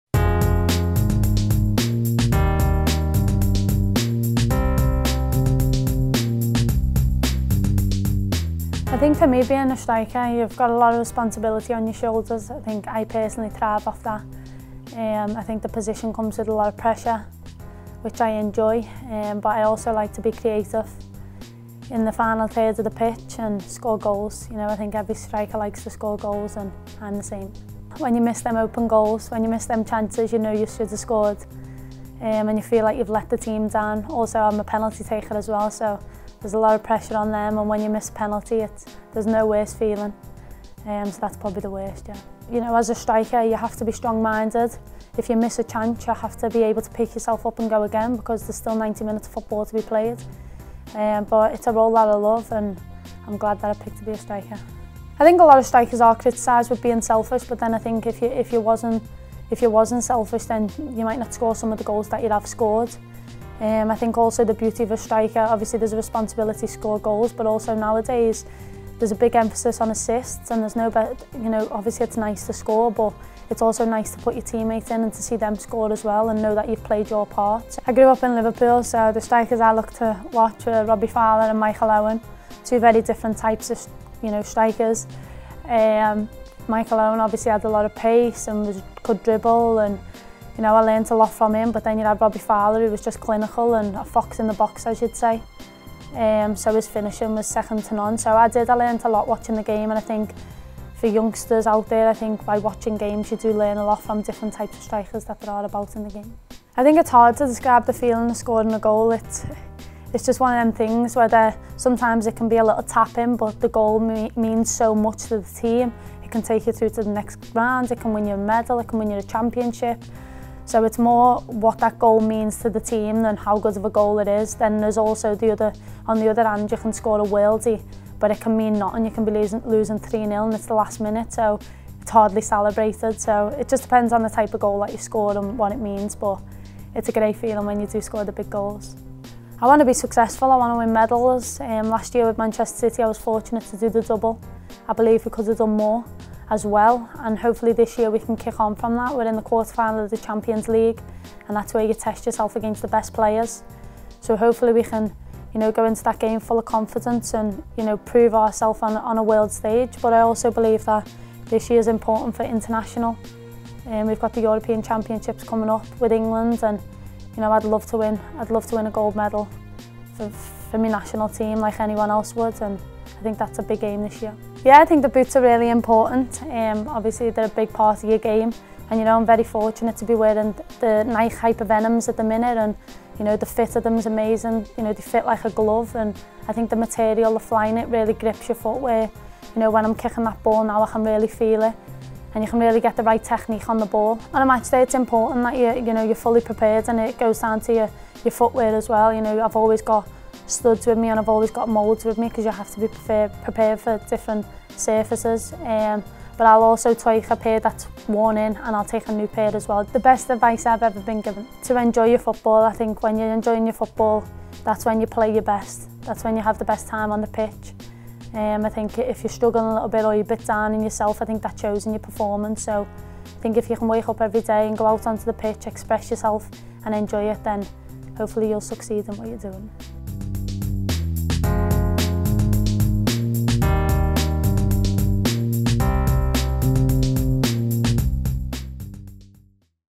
Toni Duggan | Inside the Mind of a Striker
We find out what makes the MCWFC and England striker tick

Toni Duggan wears the Nike Hypervenom Phantom 3.

Manchester City Women play Fortuna Hjørring in the Champions League in the Women's Champions League quarter-finals on Thursday.